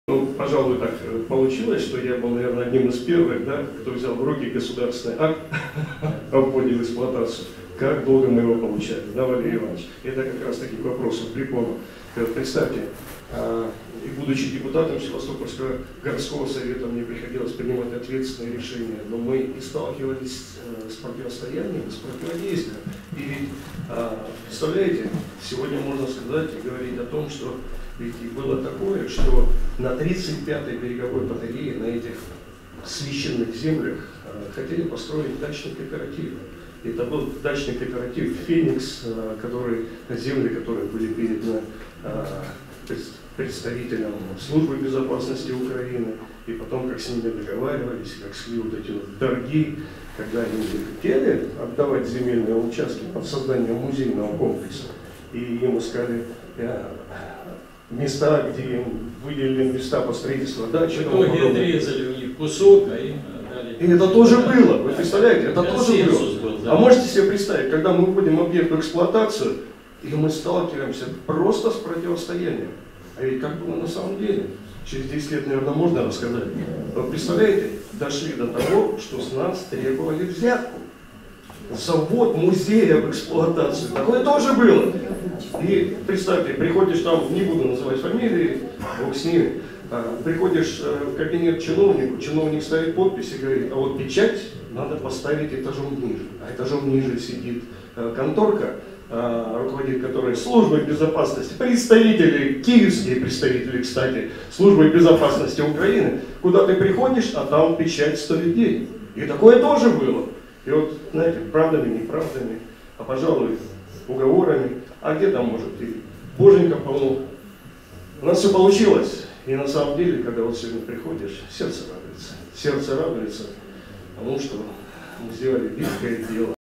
Как СБУ требовало взятку с 35 береговой батареи - откровения экс-депутата Севастополя
А ещё как Служба безопасности Украины распилила земли будущего музея. Вспоминает экс-депутат Севастопольского городского совета при Украине Андрей Семенов.
"35 береговая батарея" в Севастополе, 10-летний юбилей. Воспоминания участников создания музея, (отрывок).